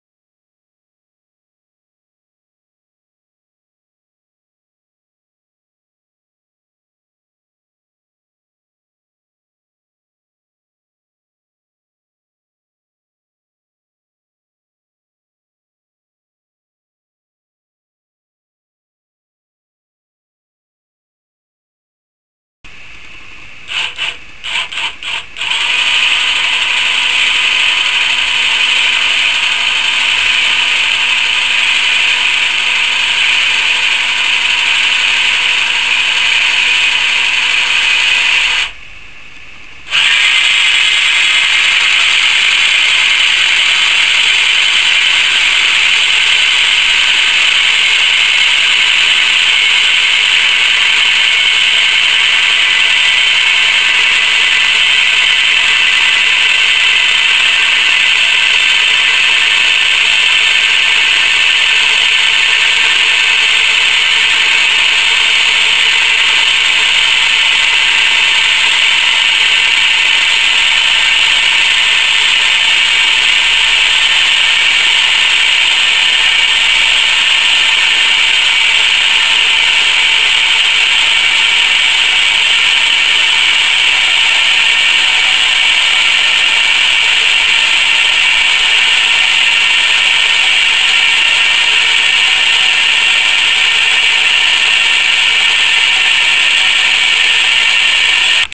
Modified Car Jack to lift a Person
Designed to lift a person, who doesn't have the use of their legs, into a car.